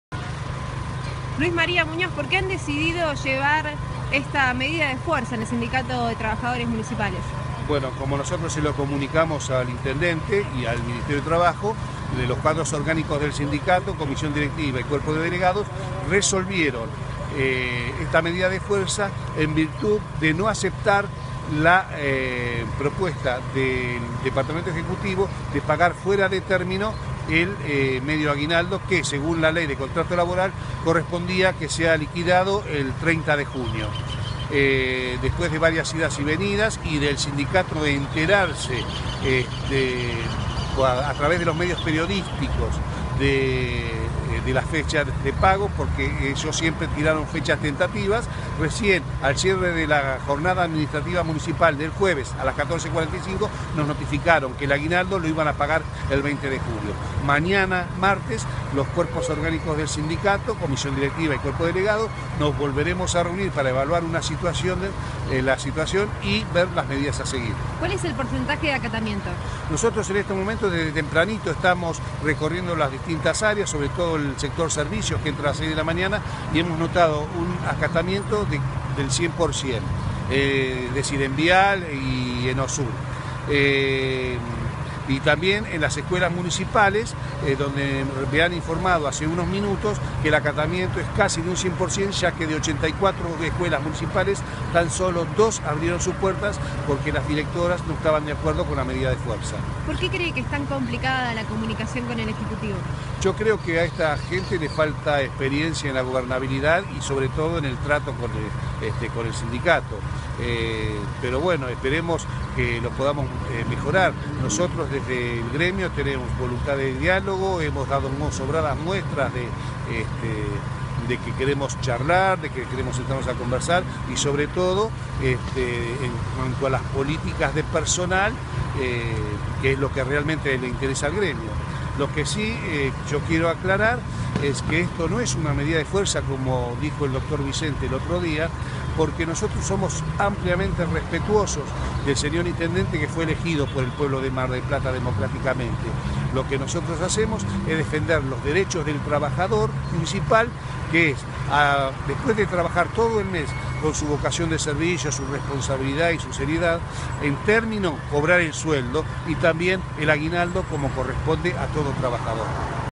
Luis María Muñoz, prensa STM. Paro Municipales
El secretario de Prensa del Sindicato de Trabajadores Municipales explica los alcances de la medida de fuerza. Aguinaldo.